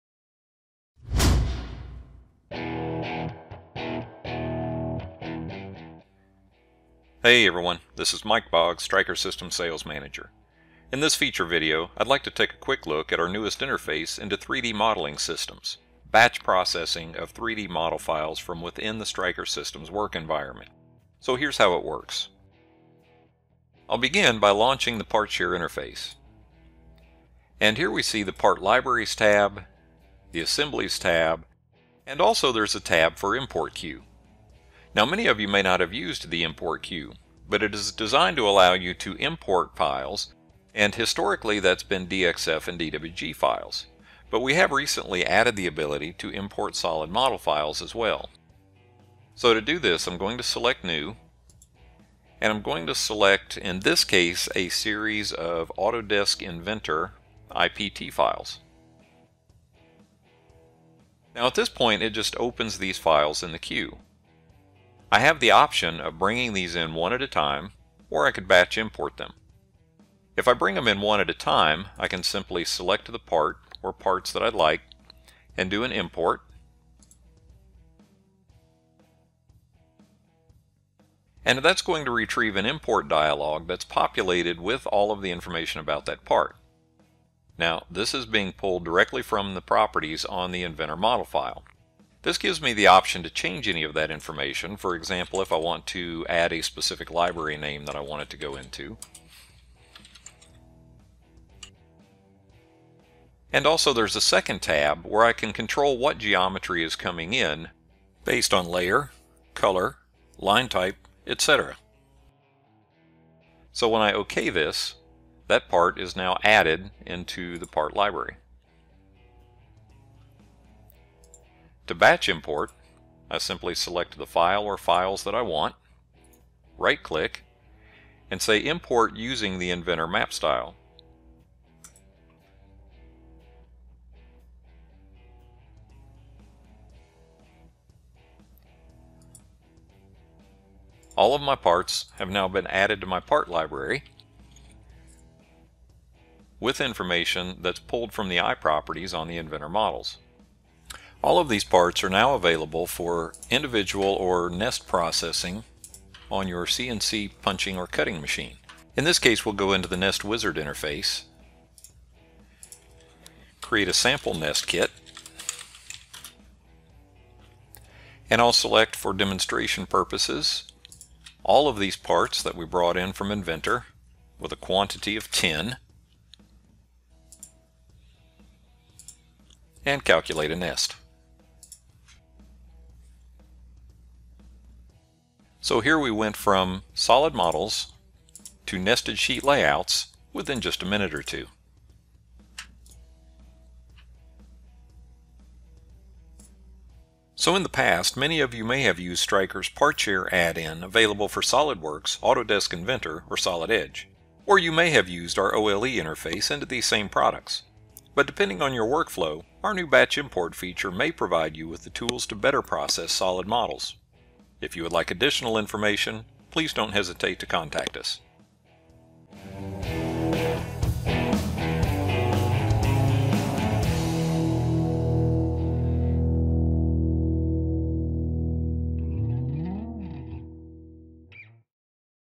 STRIKER CAD/CAM Batch Import of Sheet Metal Solid Model Files
STRIKER CAD/CAM now offers batch importing of unfolded (flat) sheet metal parts from 3D solid models for single part and nesting operations on your CNC cutting and punching machines.  Watch this video of Autodesk Inventor sheet metal parts being imported and nested.